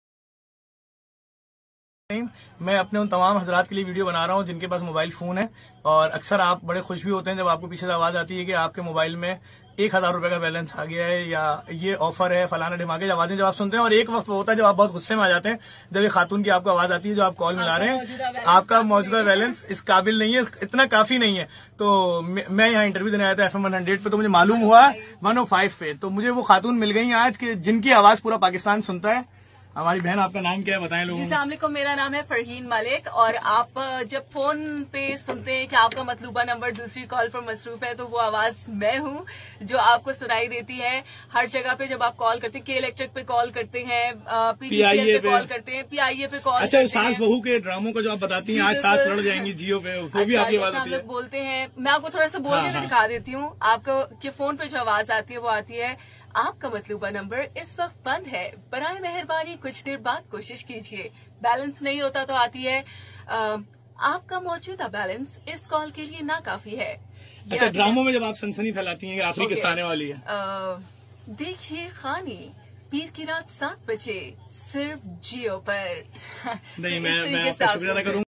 Famous voice Actor Which speak Your Balance is not sufficient for this call,prepare by Unique Tv
Interview with 100 FM anchor person by Unique Tv